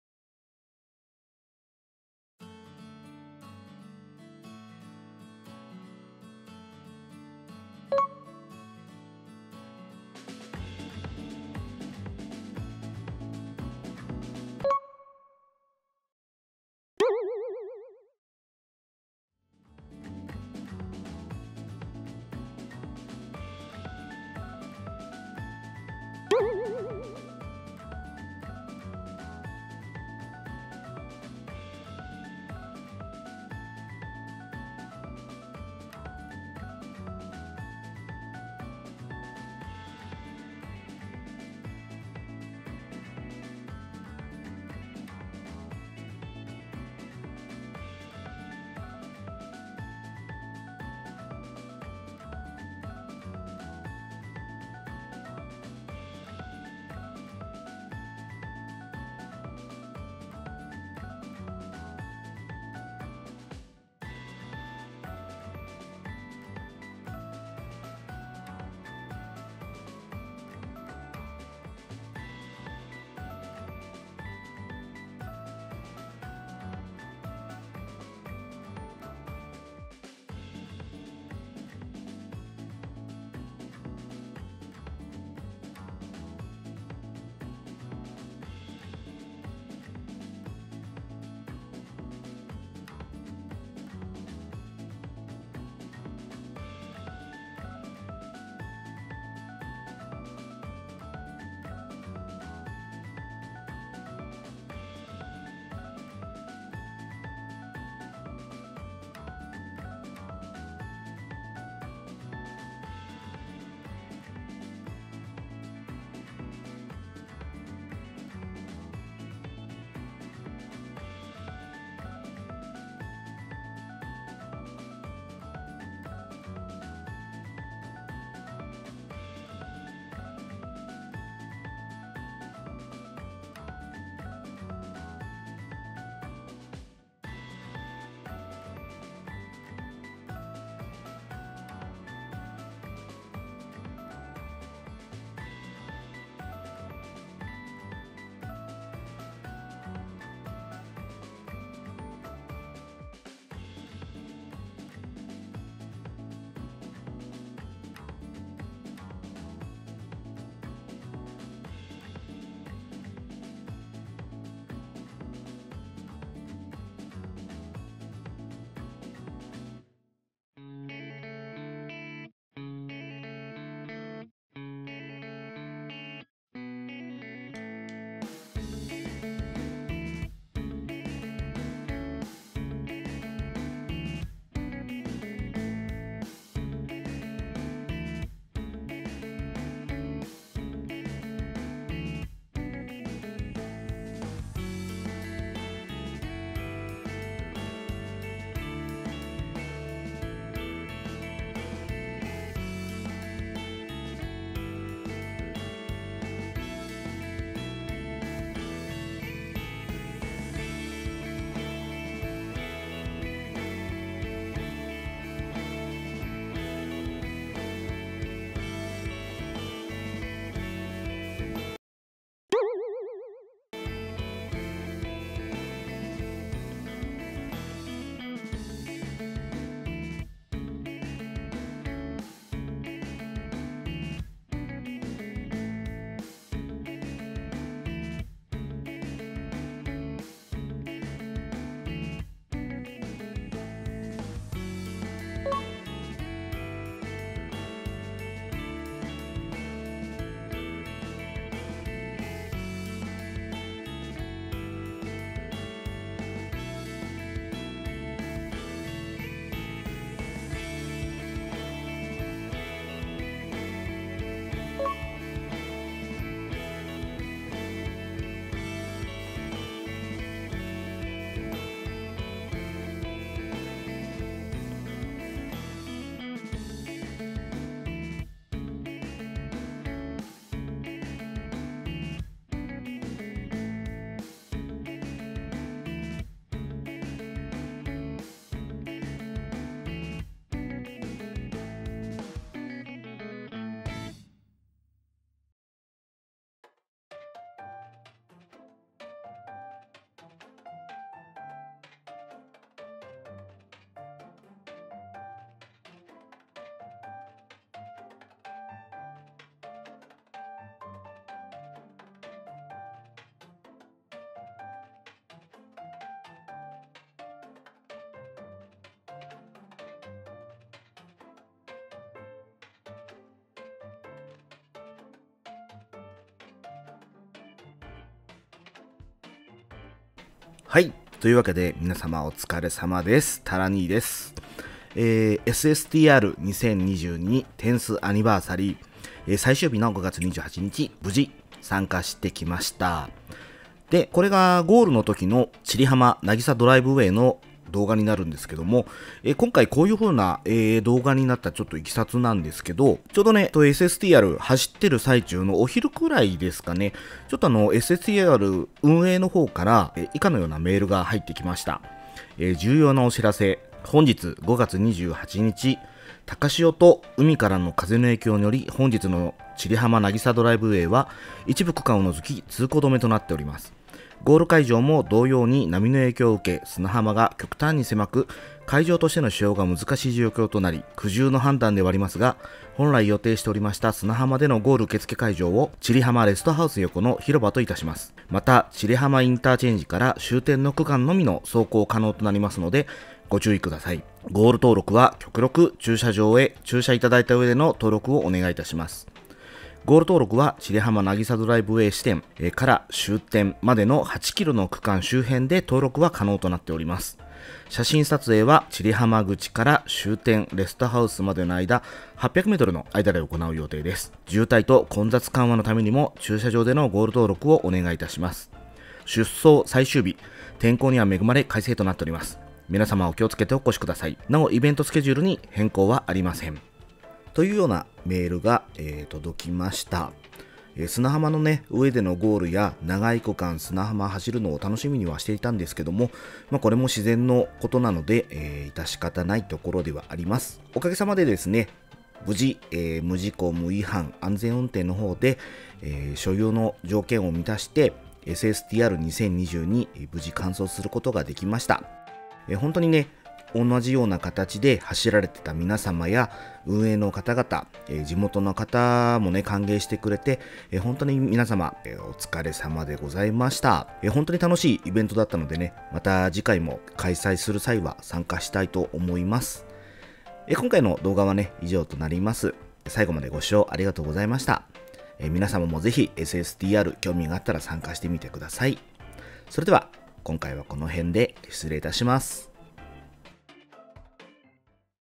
SSTR 2022 10th ANNIVERSARY 最終日5月28日 いつもと違う千里浜なぎさドライブウェイとゴールの様子
初参加したSSTR2022 最終日5月28日、ゴール時の千里浜の様子です。
最終日にまさかの高潮で砂浜のゴール撤去＆砂浜走行区間の大幅縮小という自然の影響を受けました。

少し残念でしたがSSTRのイベントは素晴らしく楽しかったです！
2022年は10月に10周年のプレミアムSSTRもあるみたいですよ。